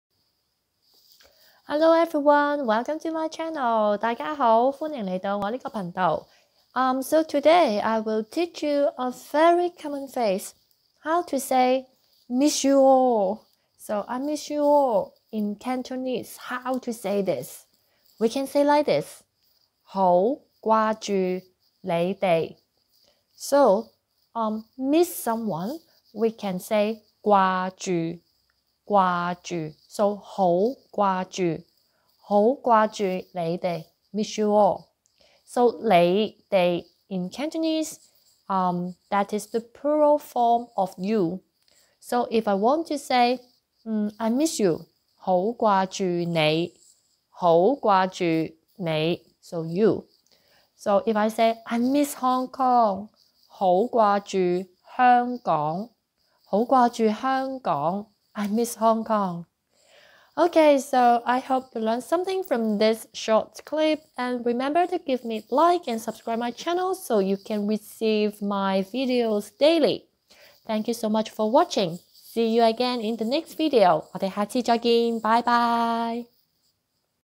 Cantonese Learning 225: How to say "I miss you all" in Cantonese.
Btw, I really want to connect with my subscribers more and if you want to have Free Online Cantonese Chatting Time with me, we could definitely arrange it.

To connect more with me:
Here are my 3 different IG accts:

winniecantonese
bgwinniemak
bgwinnie_art

I miss you all.

hou2 (very) gwa3-jyu6 (miss) nei5-dei6 (You in plural form)
好掛住你哋。

I miss you (singular You).

hou2 gwa3-jyu6 nei5.
好掛住你。

I miss Hong Kong.

hou2 gwa3-jyu6 Heung1-Gong2.
好掛住香港。